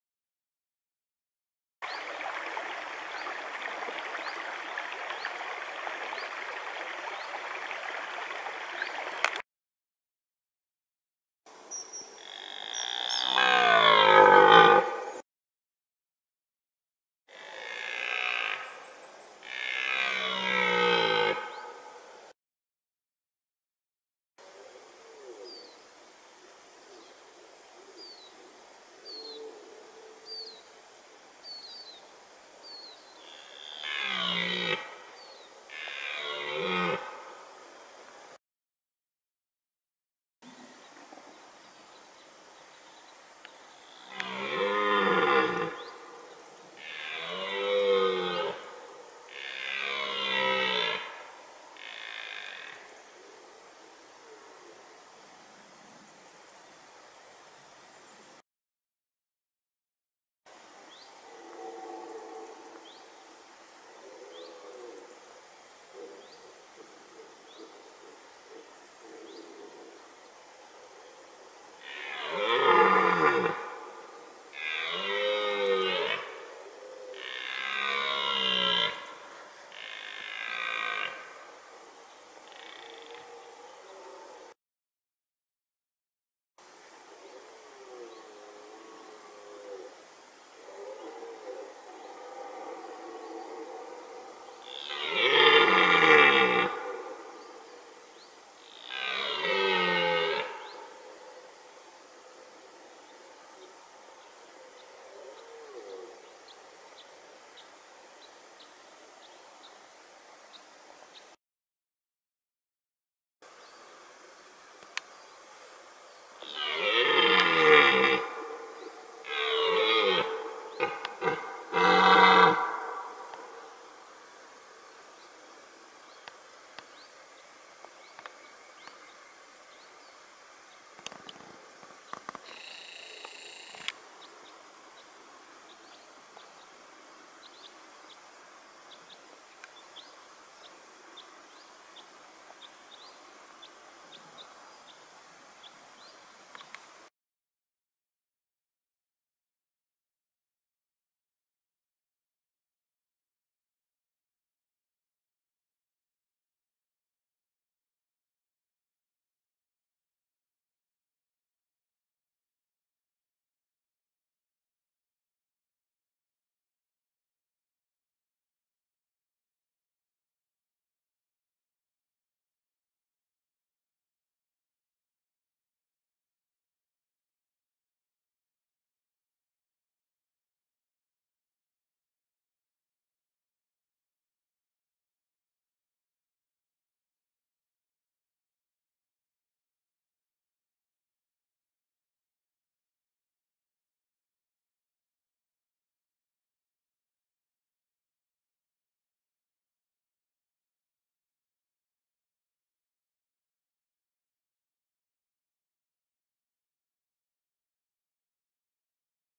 Brunfterlebnis vom 20. September 2018
Sony HX60. Beobachten ohne zu stören, mit wachem Geist gelingts. Wiederholt reagiert der Rothirsch auf das Röhren von Rivalen/Jägern? mit eigenen Rufreihen. Jeder Hirsch klingt anders. Gut zu sehen: Die Ohren bewegen sich unabhängig voneinander. Ansässige Vögel: Tannenhäher u. Wasseramsel.#